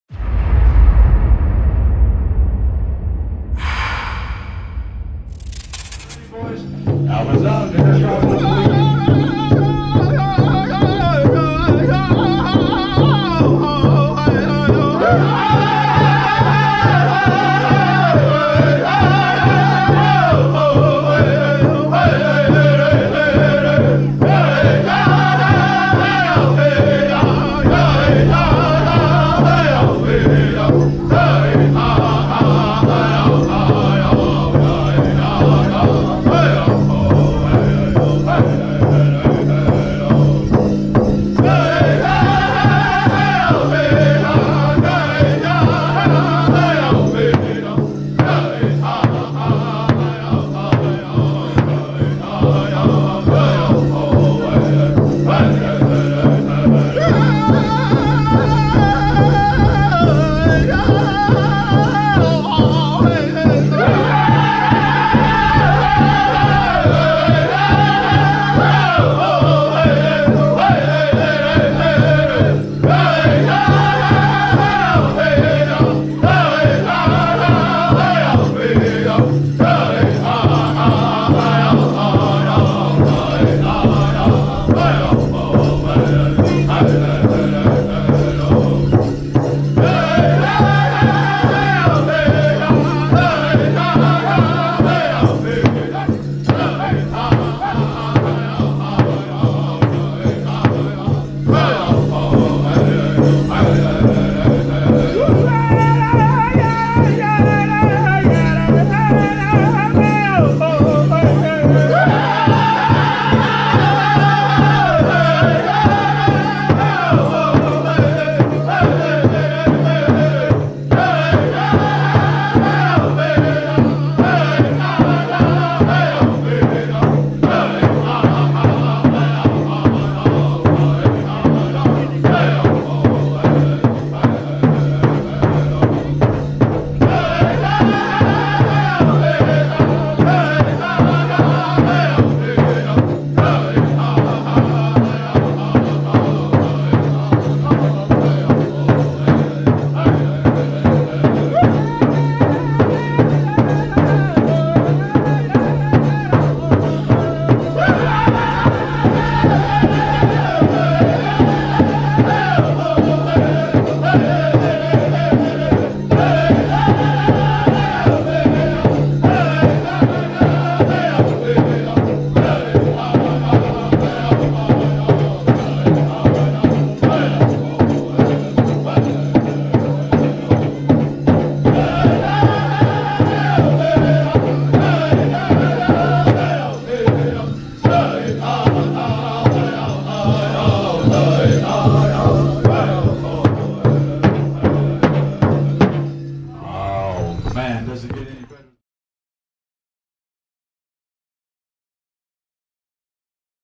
Wooden Face Singers With A Wonderful Melody | Powwow Times
Wooden Face Singers With A Wonderful Melody @ Sioux Valley Powwow 2018